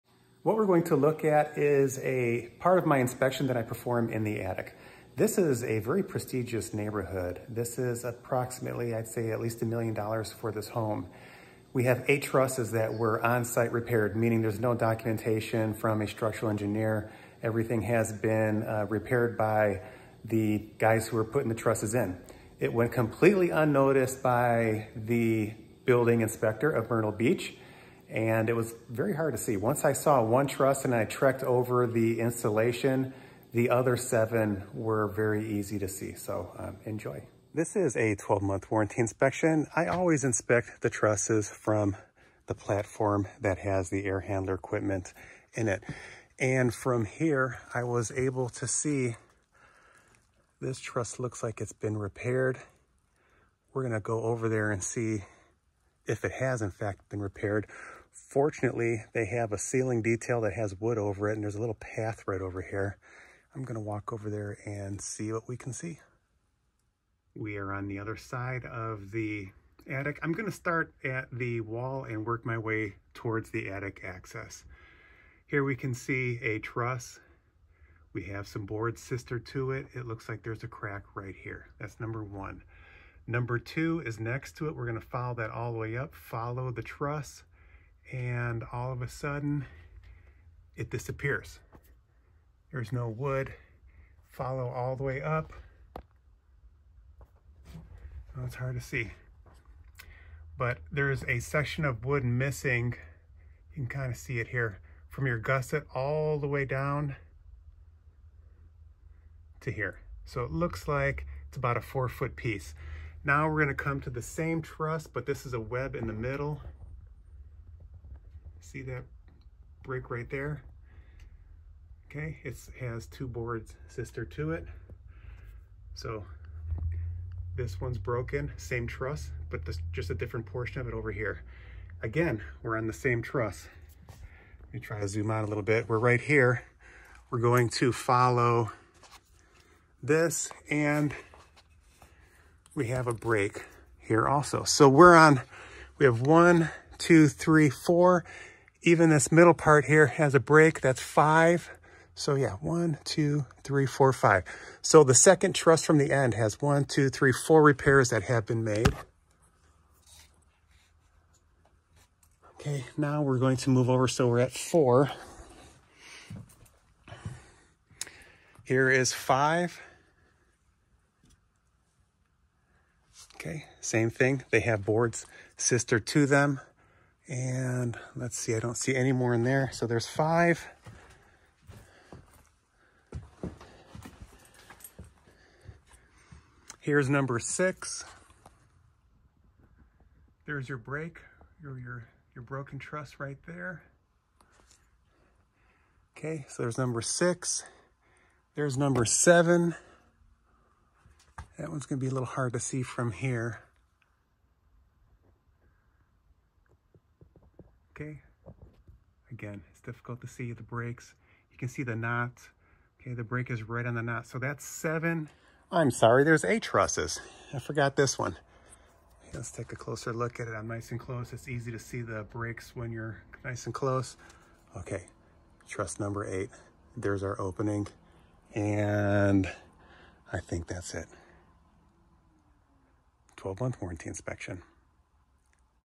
5 broken trusses with one truss having 4 breaks
￼ This is a 12 month warranty inspection in a very prestigious neighborhood in the grand dunes subdivision located on the intercoastal waterway of Myrtle Beach. During my inspection, I noticed one truss was broken. As I crossed over the attic, a total of eight breaks in the trusses were identified.

A structural engineer will need to come up with a repair method. A licensed contractor will need to make the repairs. Estimated cost of repairs was close to $3800. ￼